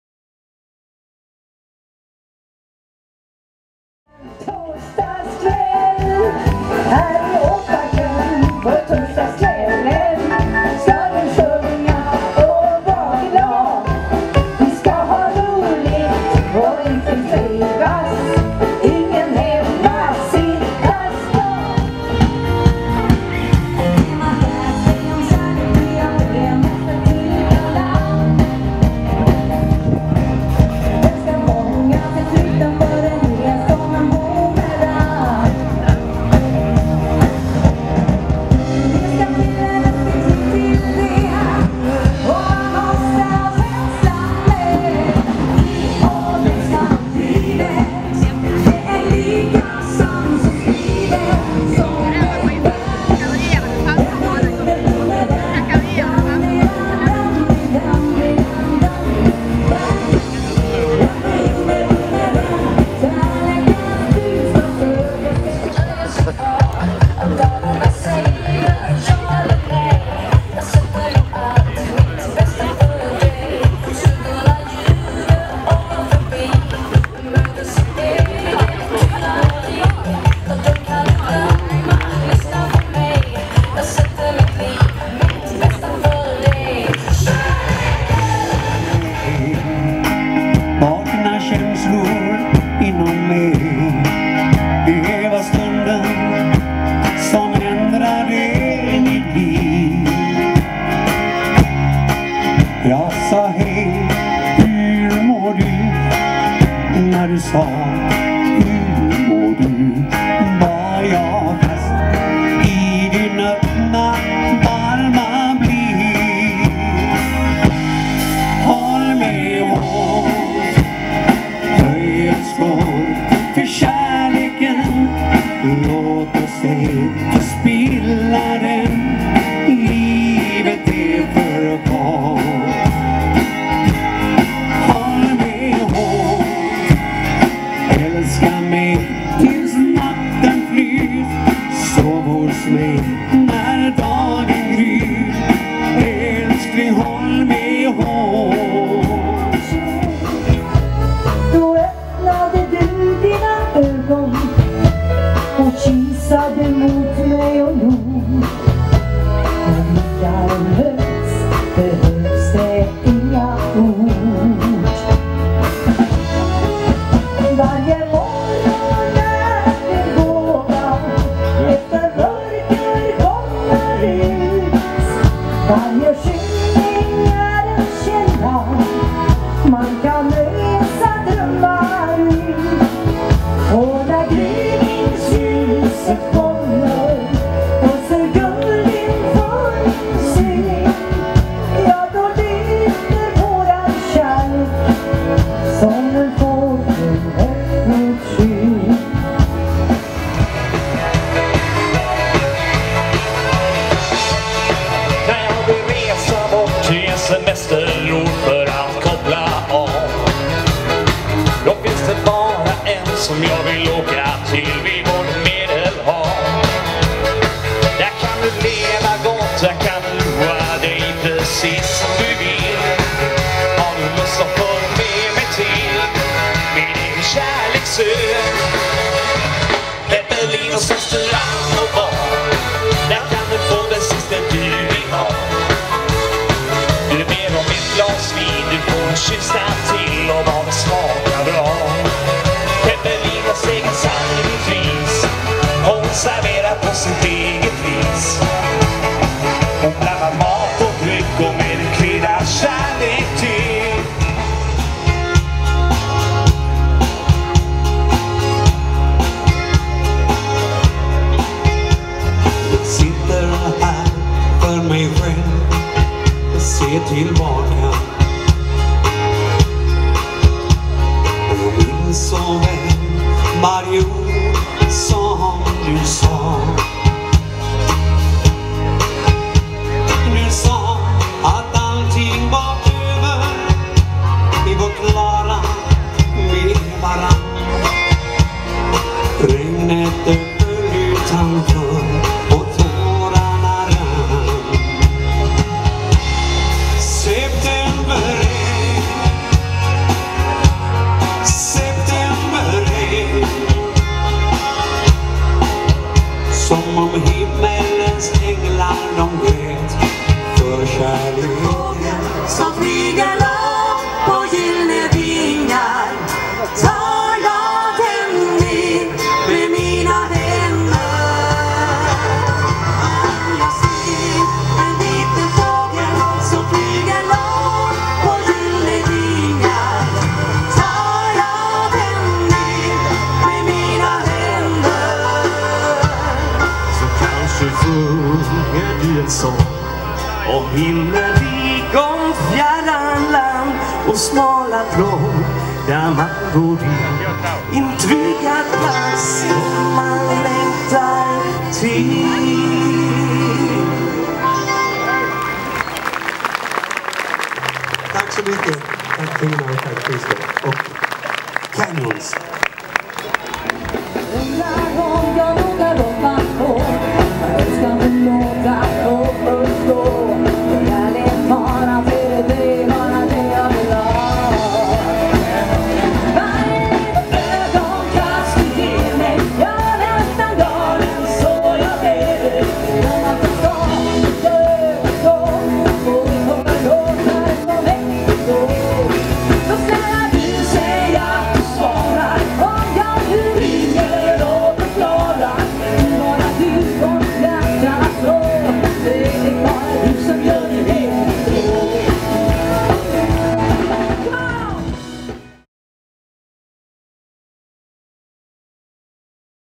Åparkskvällen med Lasse Sigfridsson, Tyresvingen, Wrambjers
Den andra Åparkskvällen blev också lyckad med fint väder och mycket folk. Gästartisterna var Dansbandet Canyons med Tina Svensson och Lasse Sigfridsson. 11 juli 2013 i Tyringe